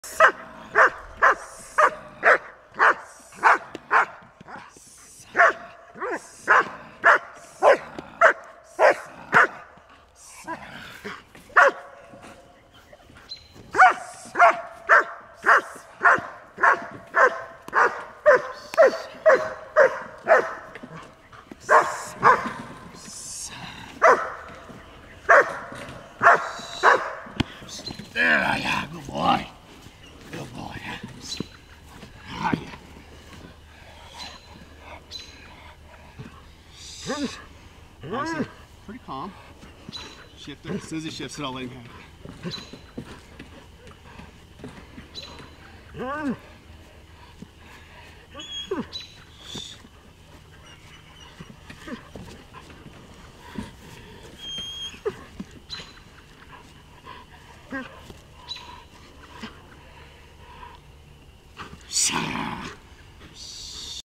Rasky Anrebri, Czech Import, German Shepherd Dog Bitework
Rasky has not done any bitework since 2008.  This video was taken June 21, 2011.